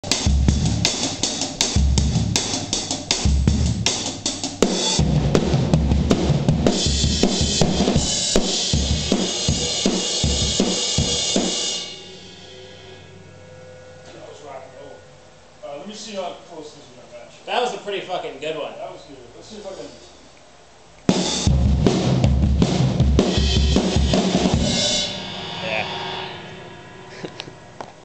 Jeremiah Nelson and the Achilles Heel - Drum Tracking 5/27/09
Adam Cargin tracks drums for the upcoming Jeremiah Nelson and the Achilles Heel record at jchristopherhughes' studio in Milwaukee, WI.